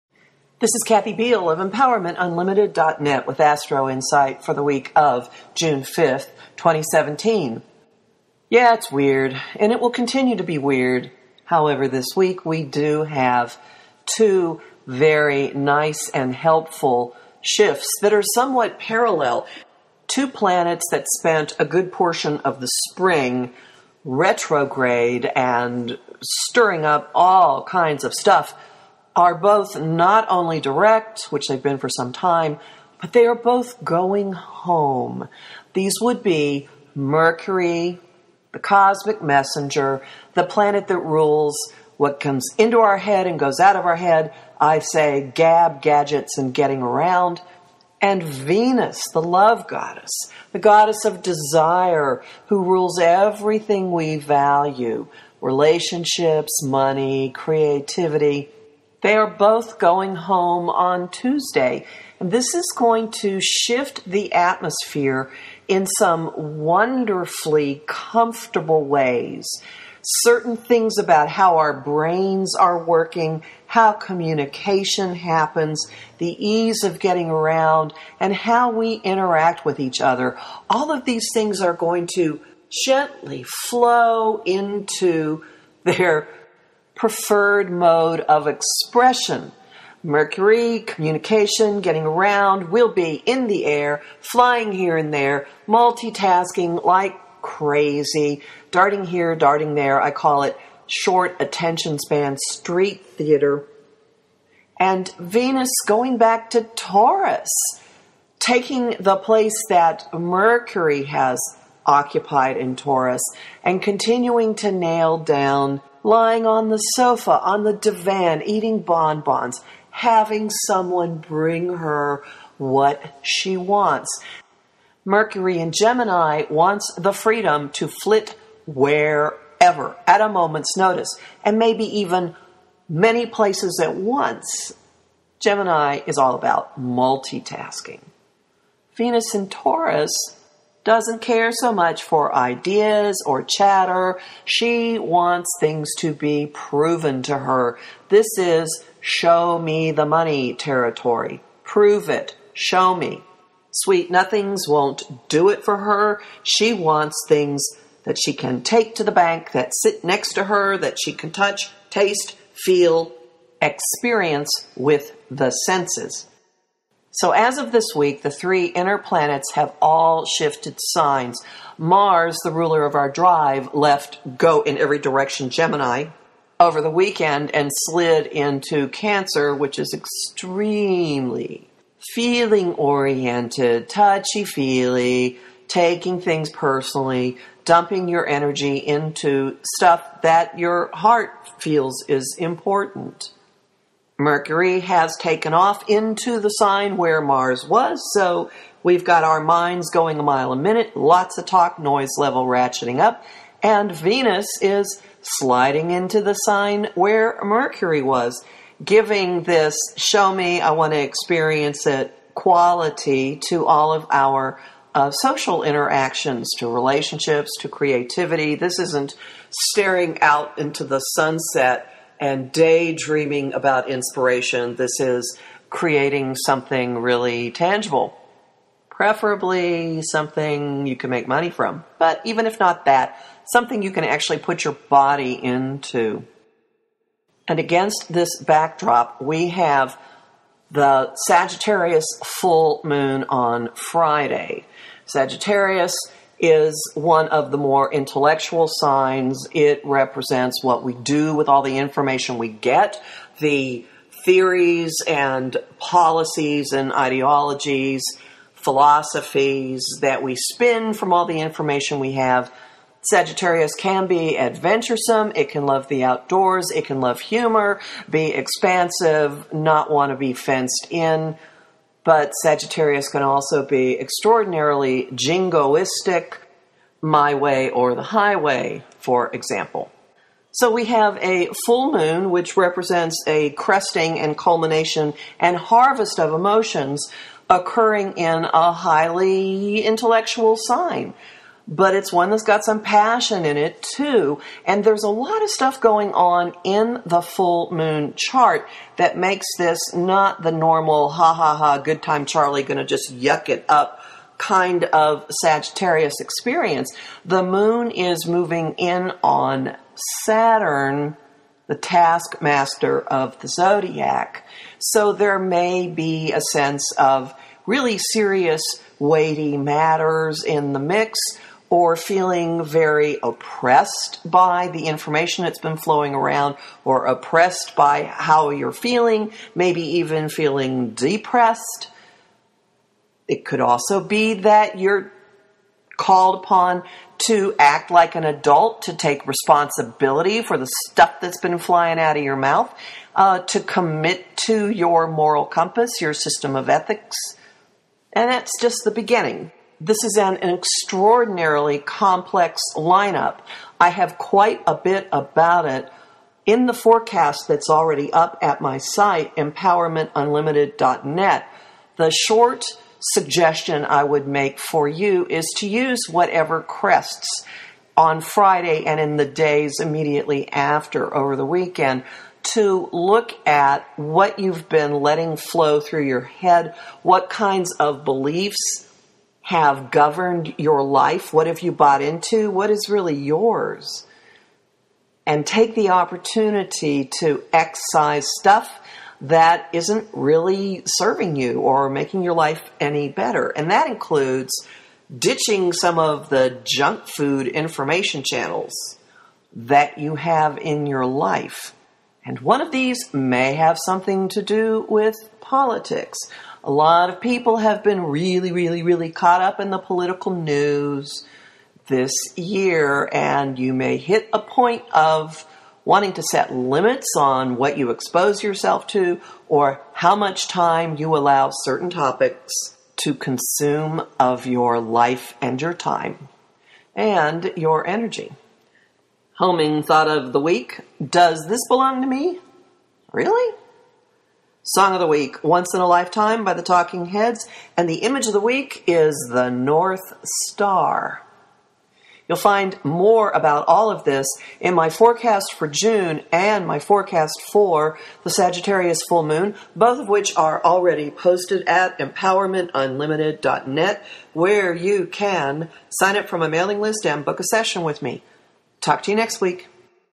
Astro Insight for June 5-11, 2017
Mouths and hearts are back in working order, at last, as Mercury and Venus go home. Good thing; the reach won’t be too far to pull out big-kid pants, requisite attire for June 9"s  Sagittarius Full Moon.

Kathy Biehl's light-hearted but practical forecast for June 2017